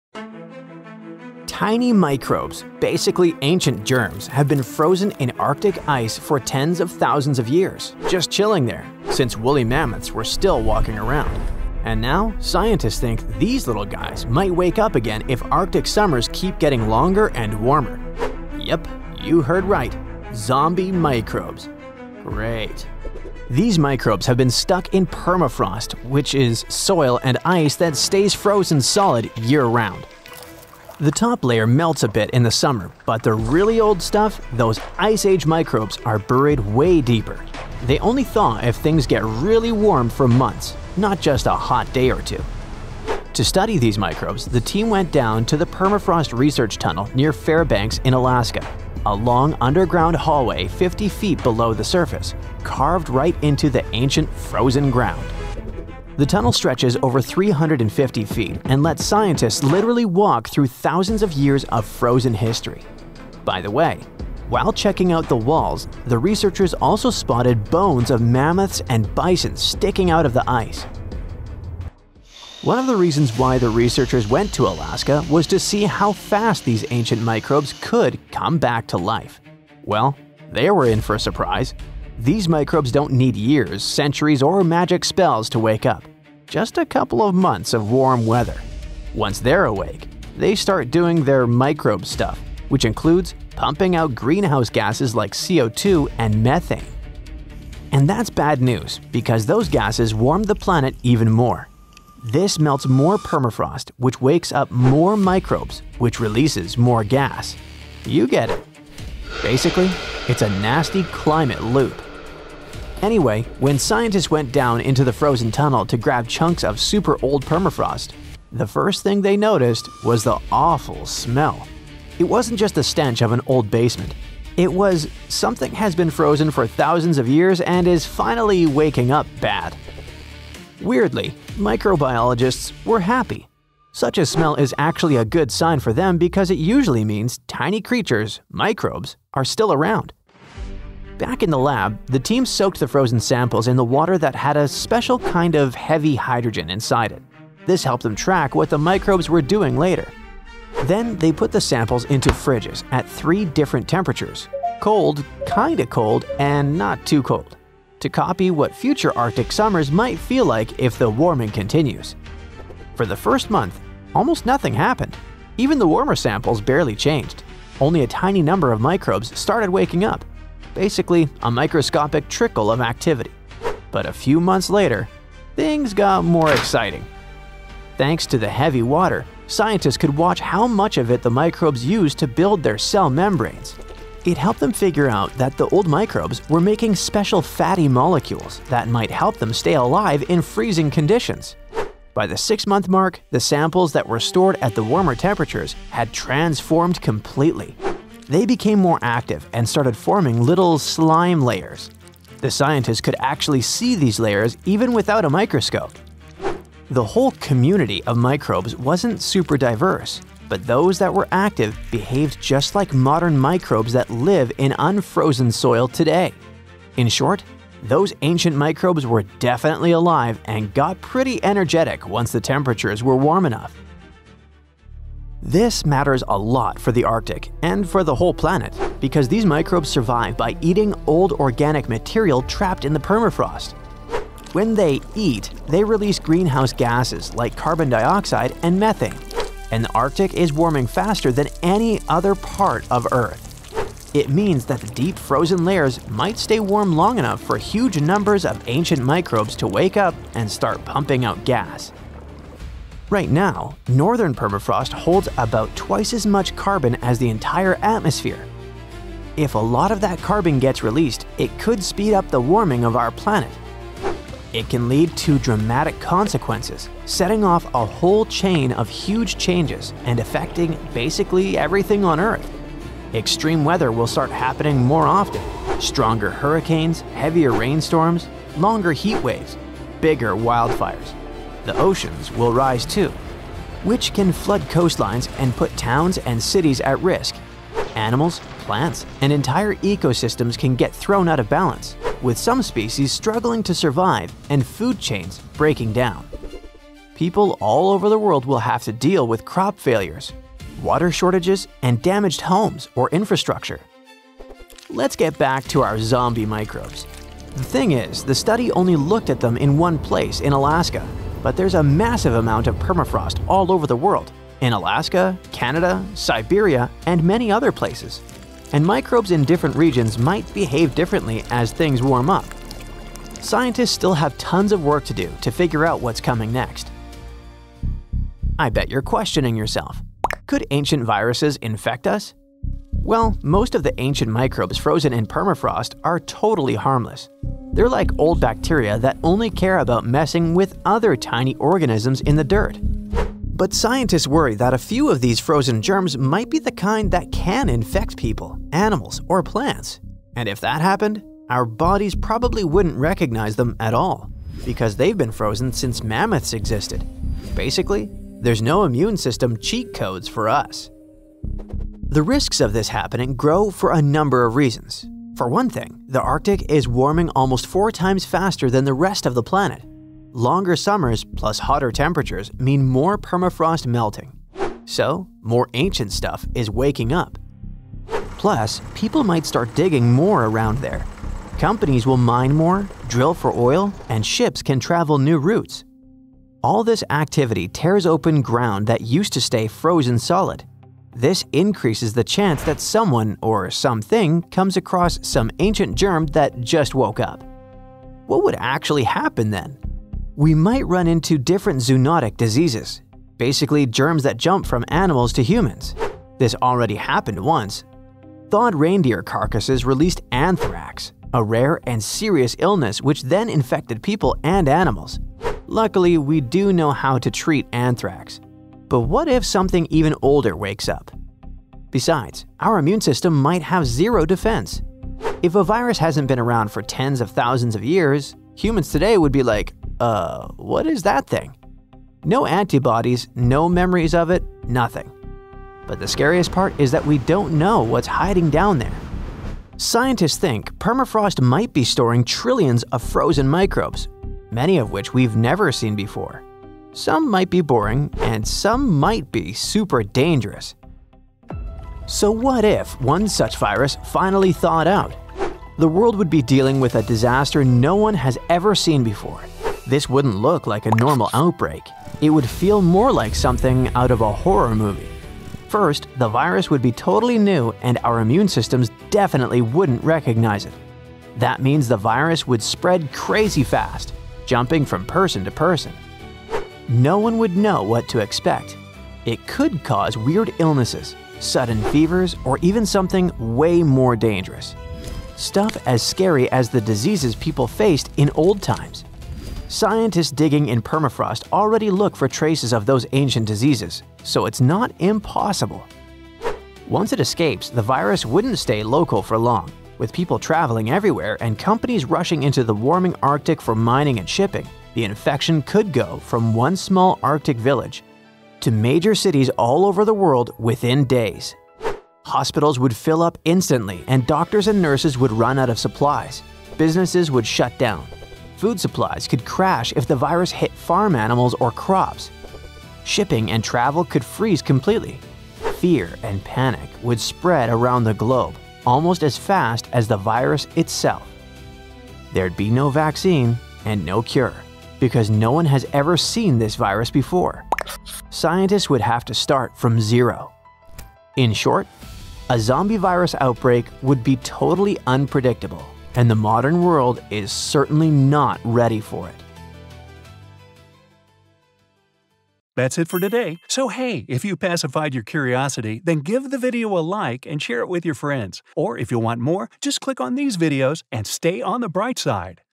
Ancient Microbes May Be Waking Up in Alaska, Scientists Warn
🧊🦠 Scientists are warning that ancient microbes buried deep in Alaska may be waking up. As frozen ground thaws, organisms that have been dormant for tens of thousands of years could be released. In this video, we explain what scientists are finding, why it’s happening now, and how serious the risk really is. We’ll separate real science from fear-driven headlines and explain everything in simple terms. If hidden dangers and climate-driven mysteries grab your attention, you won’t want to miss this.